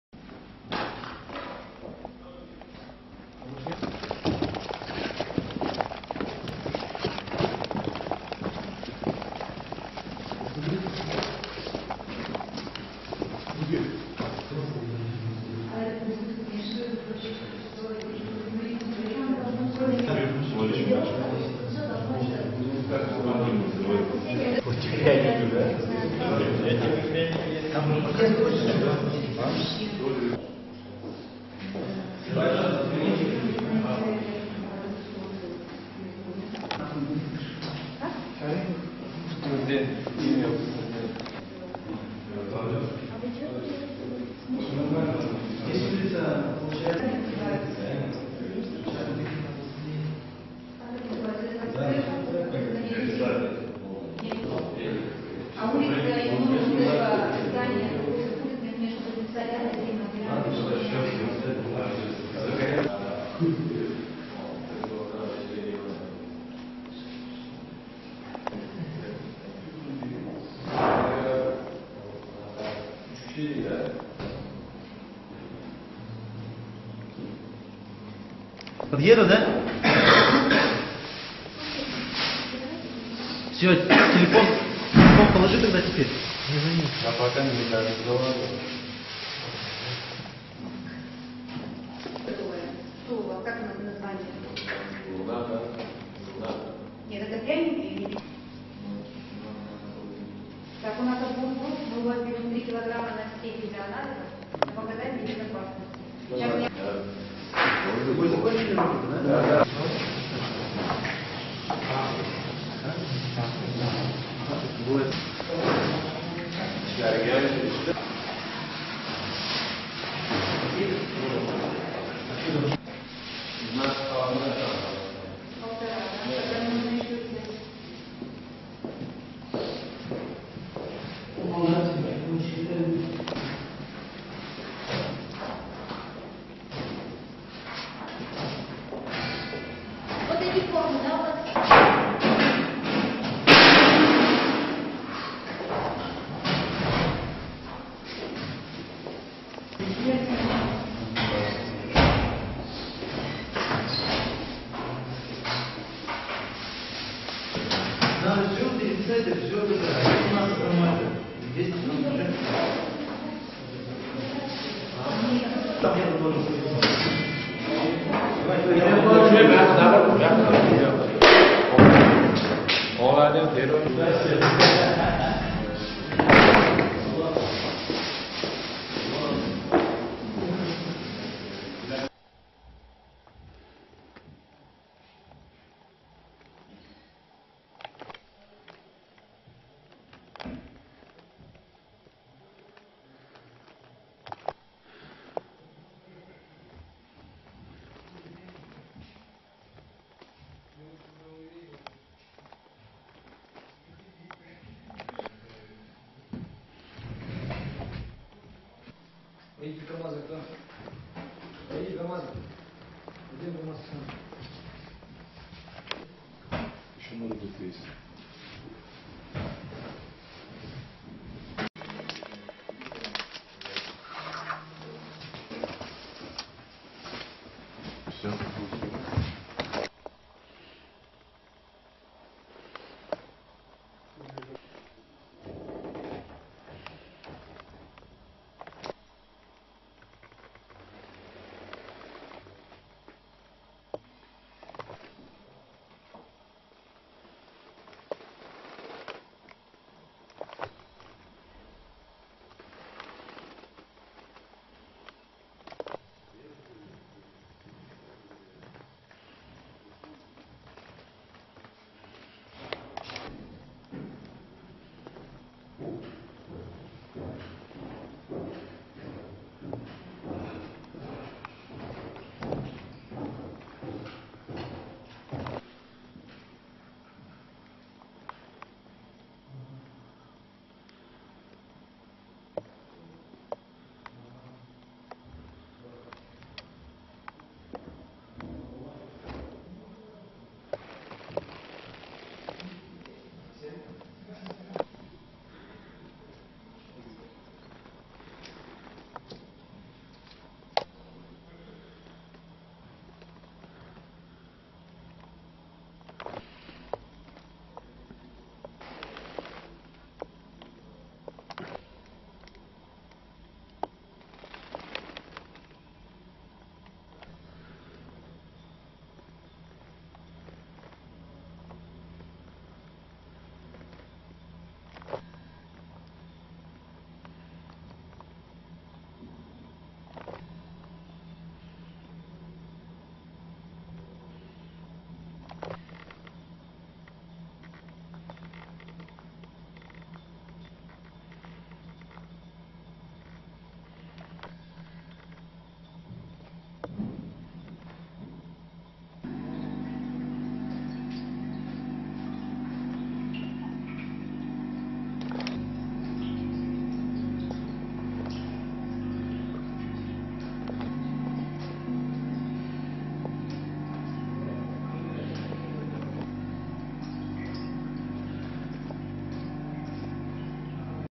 Новости Марий Эл
Сотрудниками УЭБиПК МВД по Республике Марий Эл в ходе совместных мероприятий с отделом по борьбе с организованной преступностью, СОБР МВД по Республике Марий Эл, УФМС России по Республике Марий Эл и специалистами Управления Роспотребнадзора в поселке Оршанка выявлен цех по производству кондитерской продукции, не отвечающей требованиям безопасности.